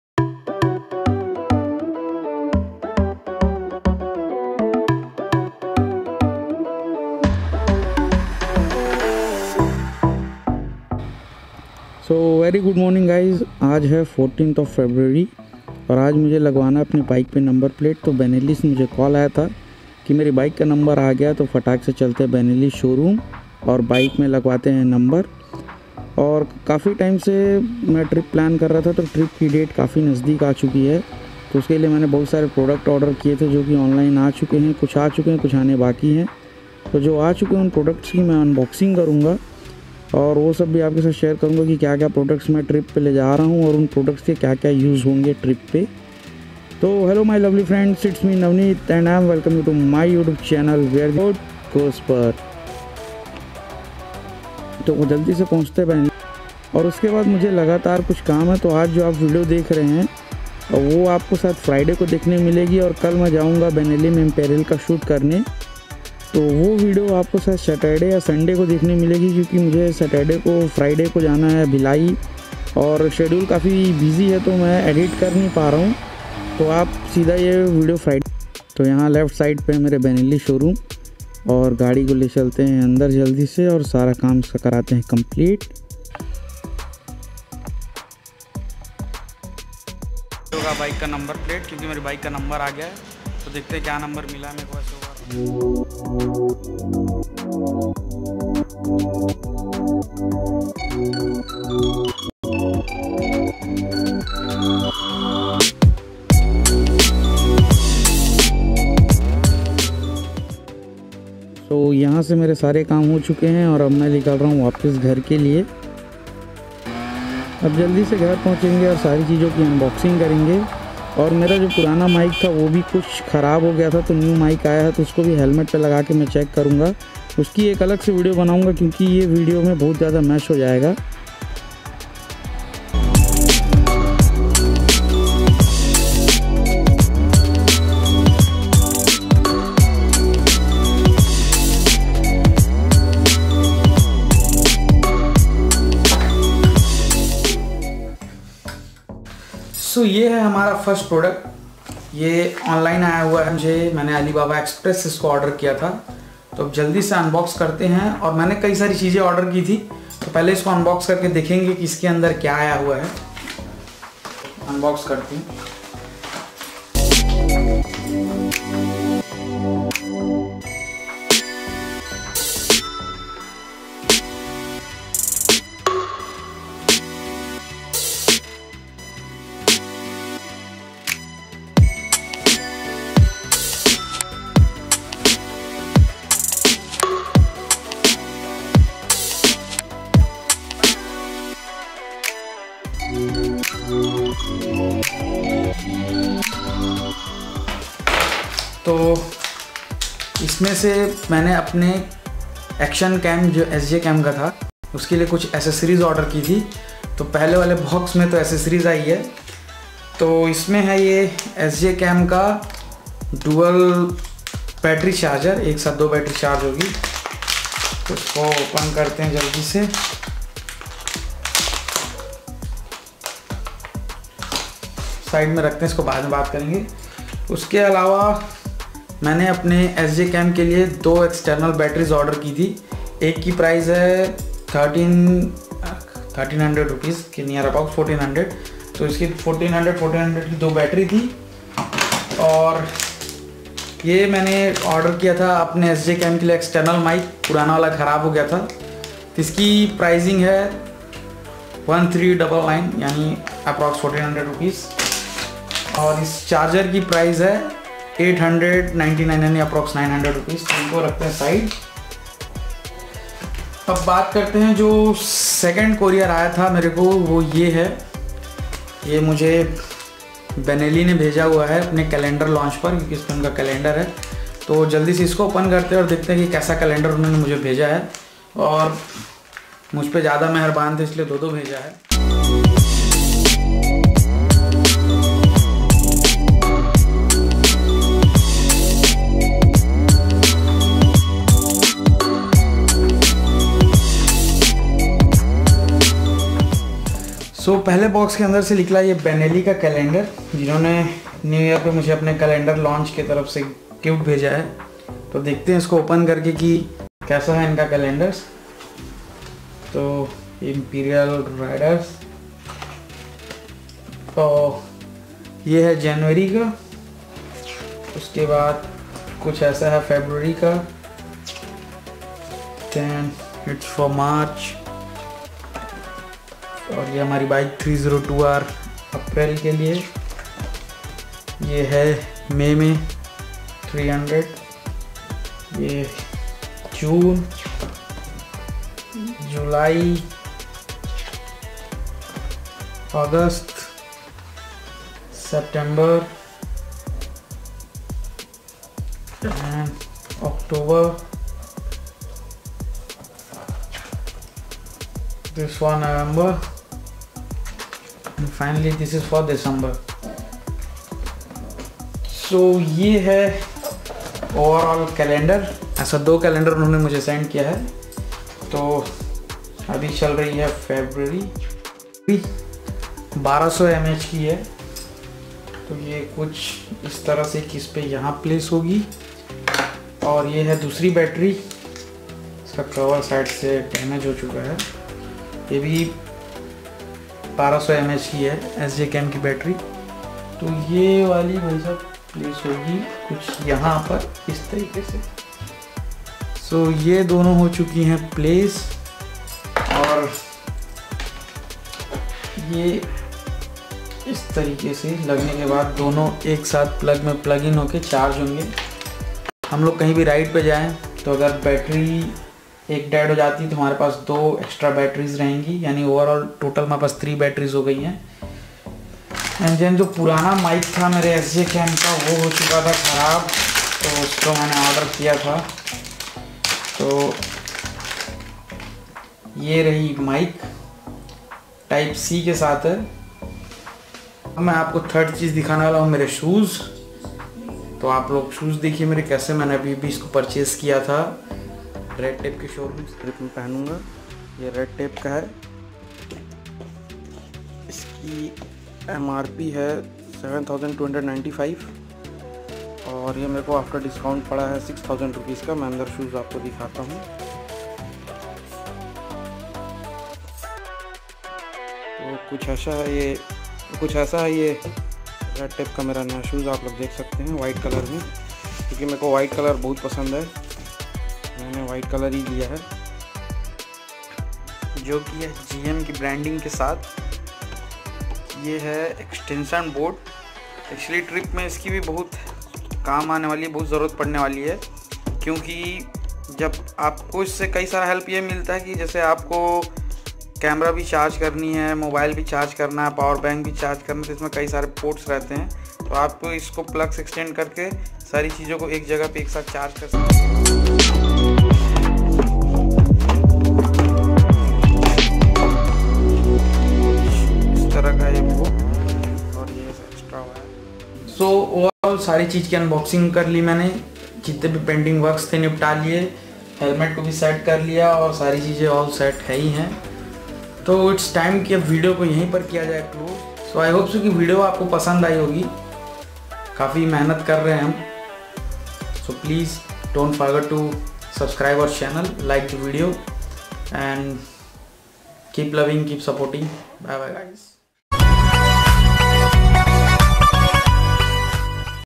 unboxing unboxing/preperation of the long ride started
video is all about the preperation of the ride in this video i unbox the diffrent products which should be get used on the ride
i ll get the number of my bike do little much of motovlog benelli sent me the calender of their calender launch or sjcam accesries which i order i get from.

video courtesy'
navneet tripathi

edit credit
navneet tripathi

background voice
navneet tripathi